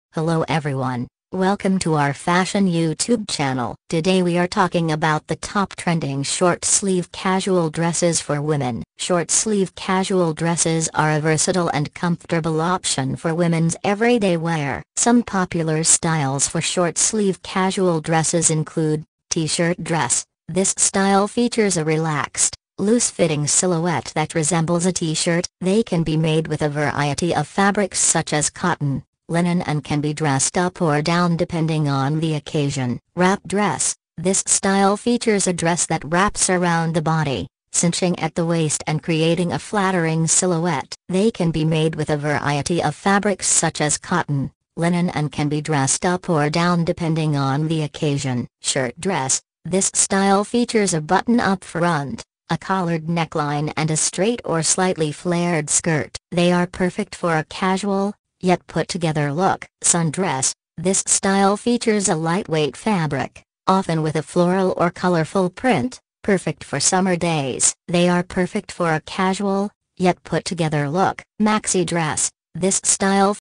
Top 10 For women's short sleeve casual dresses Ideas 2023
Short sleeve casual dresses are a versatile and comfortable option for women's everyday wear. Some popular styles for short sleeve casual dresses .
★★★★★ Trendy casual dresses
1. Alex Evenings Women's Plus Size Short Sleeve Long A-line Rosette Lace Dress
2. MEROKEETY Women's Summer Striped Short Sleeve T Shirt Dress Casual Tie Waist with Pockets
3. PRETTYGARDEN Women's Floral Wrap V-Neck Midi Dress Short Sleeve Boho Summer Beach Long Dress High Low Hem Smocked Dress Women
4. ANRABESS Women's Casual Loose Short Sleeve Long Dress Split Maxi Summer Beach Dress with Pockets
5. PRETTYGARDEN Women's Floral Boho Dress Casual Short Sleeve V Neck Ruffle Tiered 2023 Summer Swing Maxi Dresses
6. BTFBM Women's 2023 Summer Casual Beach Dresses Crew Neck Short Sleeve Wrap Party Club Mini Ruched Bodycon T Shirt Dress
7. Calvin Klein Short Sleeved Seamed Sheath Women’s Casual Dresses with Professional Flair
8. PRETTYGARDEN Women's Summer Short Sleeve Crewneck Striped Dress Basic Solid Tie Waist Office T Shirt Dresses Pockets
9. MEROKEETY Women's Short Sleeve Crew Neck T Shirt Dress Tie Waist Ruched Bodycon Mini Dress
10. BTFBM Women's V Neck Smocked Short Sleeve Casual Dress Swiss Dot Swing Flowy Ruffle Hem Mini Beach Summer Dresses 2023
Pic from Amazon